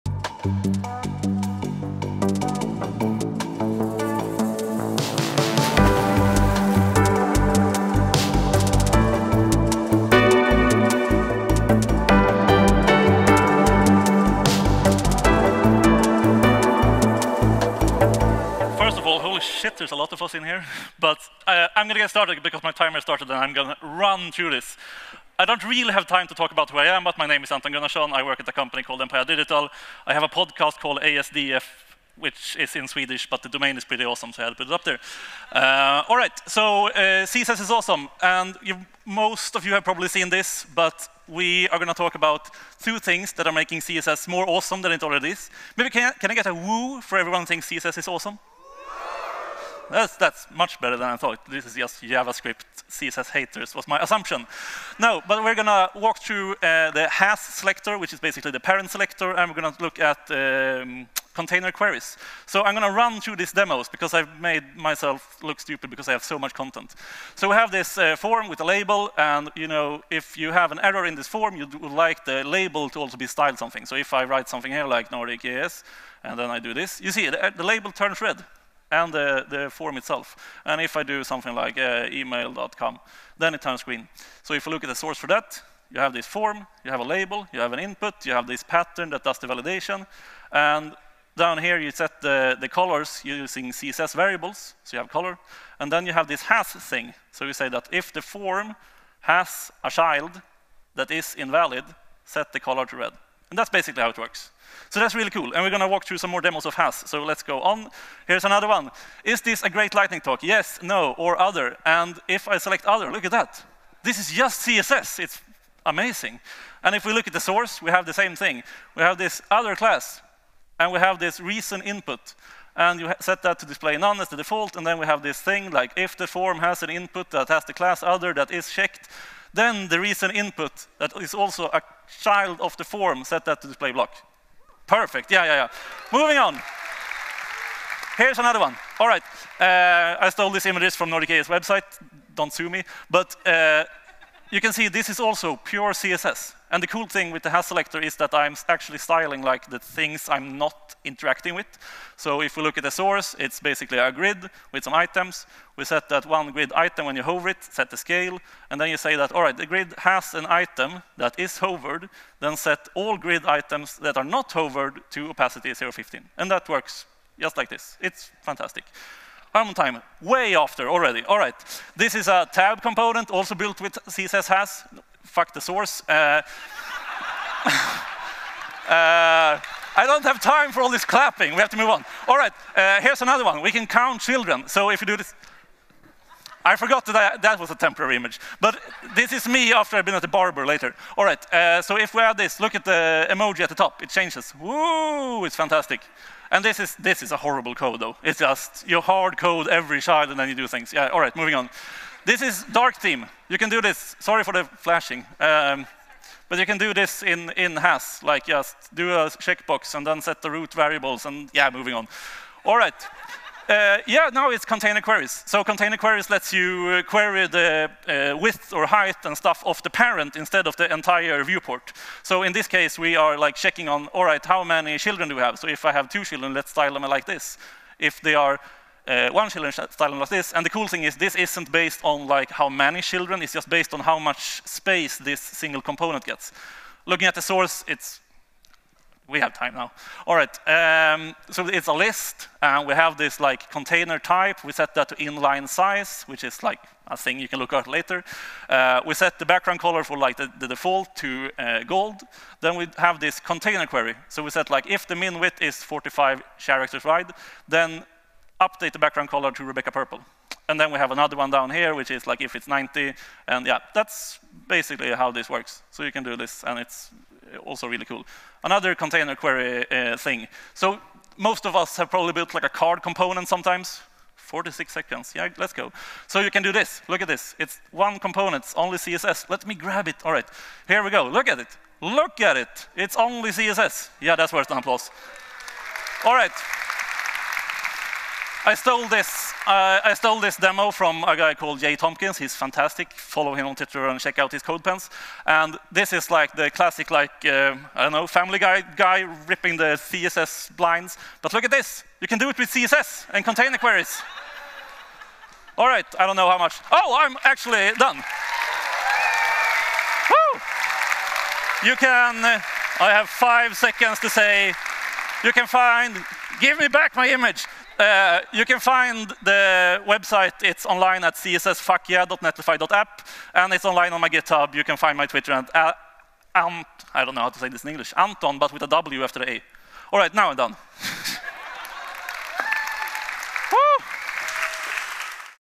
Nordic.js 2022 • Lightning talk: Anton Gunnarsson - CSS *is* awesome
Nordic.js is a single track conference with talks by internationally renowned speakers and rising stars in the JavaScript community.

Bryntum - Futurice - Columbia Road - Schibsted